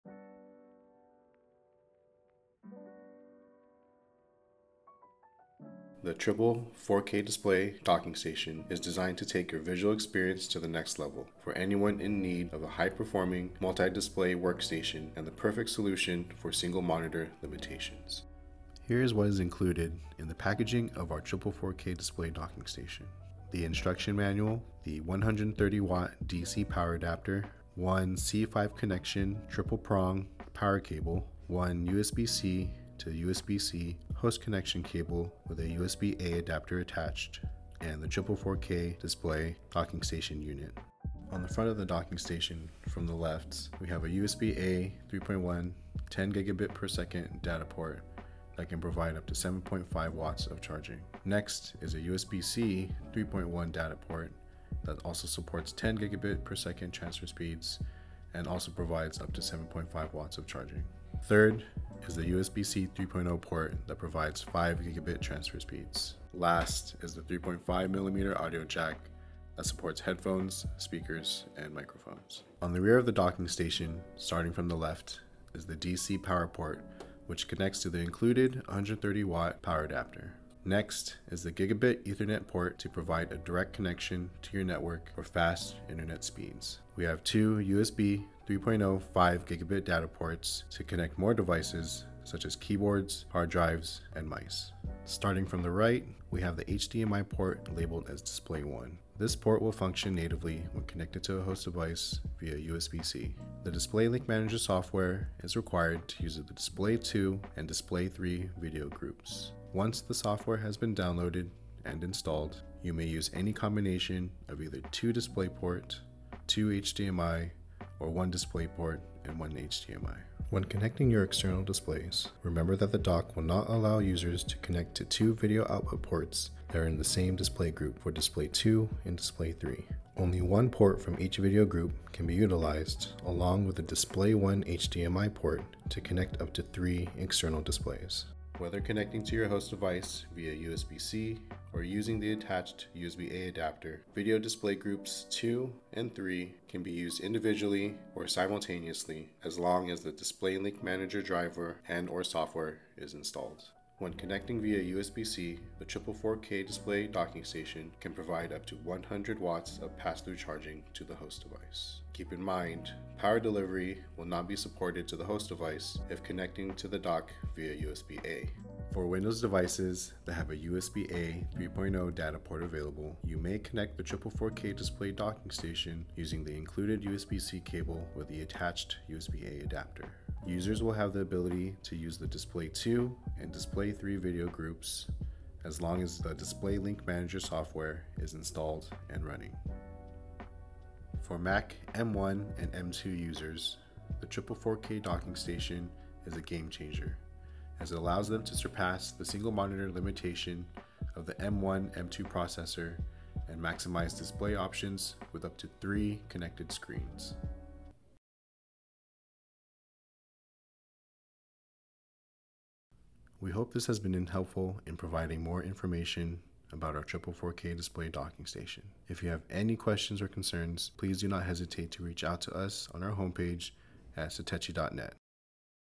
Satechi Triple 4K Display Docking Station Overview
The Triple 4K Display Docking Station is designed to take your visual experience to the next level. Truly universal, this docking station is compatible with any host device – Mac or Windows OS – that is equipped with a USB-C or USB-A port. If you are looking for a high-performing, multi-display workstation, then this dock is just what you need.